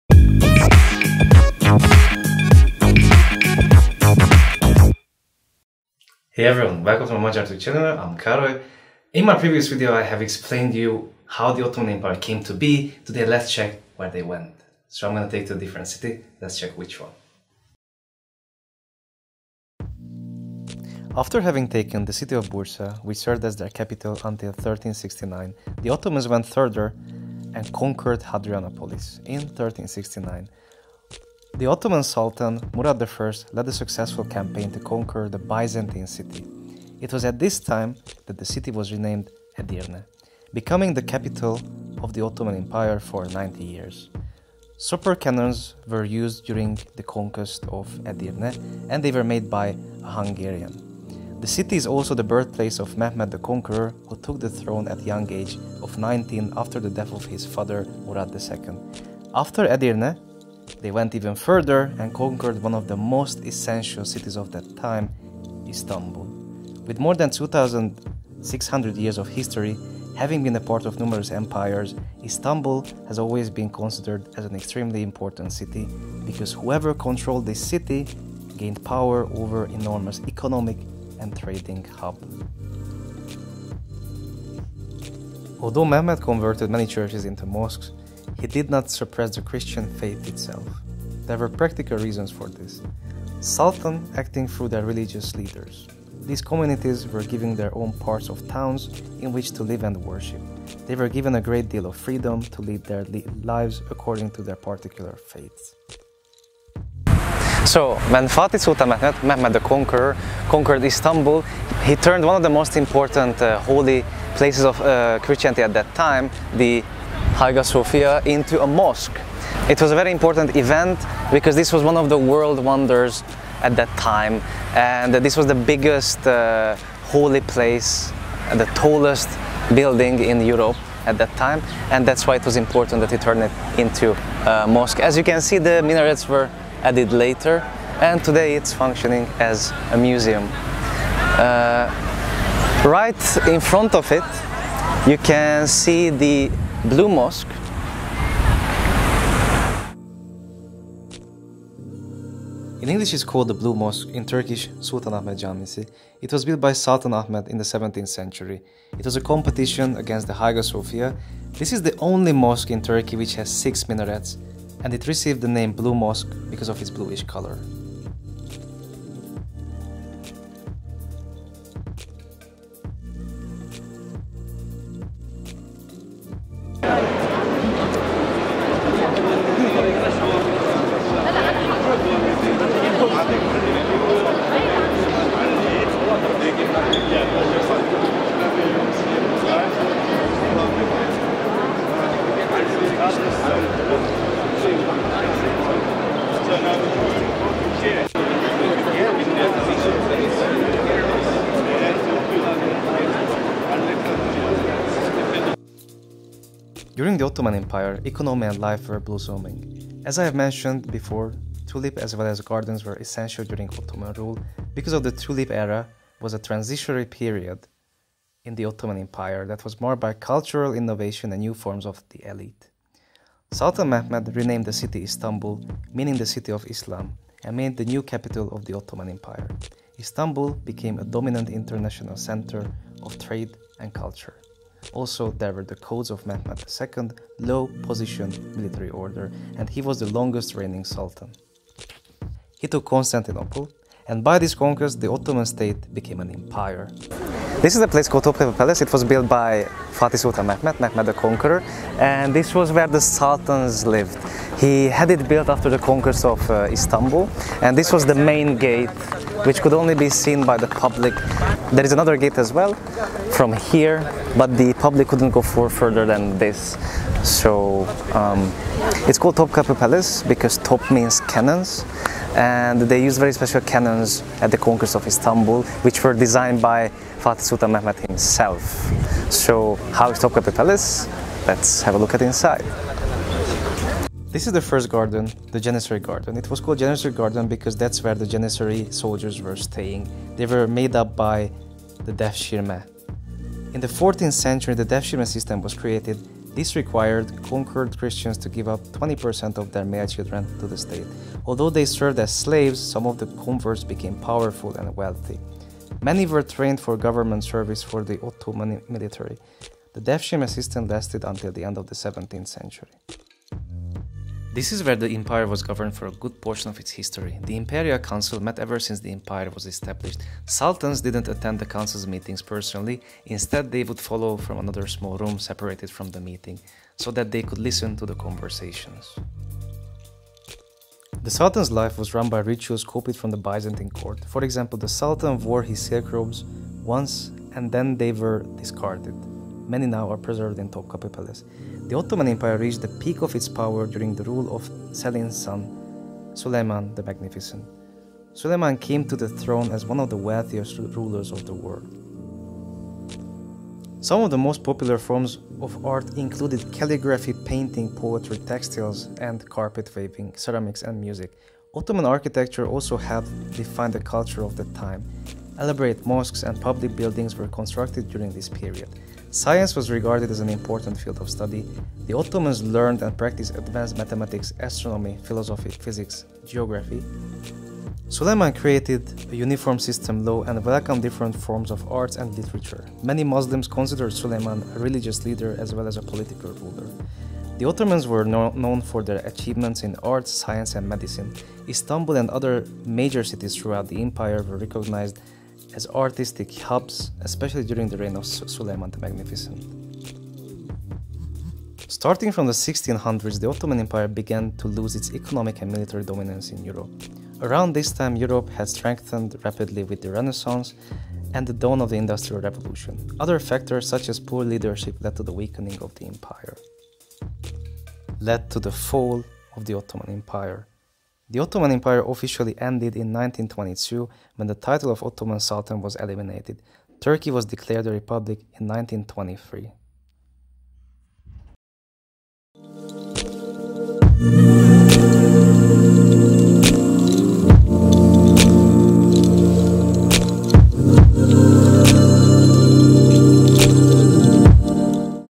The Conquest of Constantinople - Konstantiniyye'nin Fethi
In this video, you can experience the greatest victory in Ottoman history, see the importance and effects of it. Explore the palace of administration and education of that time, as well as extend your knowledge about some of the most influential sultans of the Ottoman Empire.

Enjoy:) and don't forget sharing is caring:)
AND!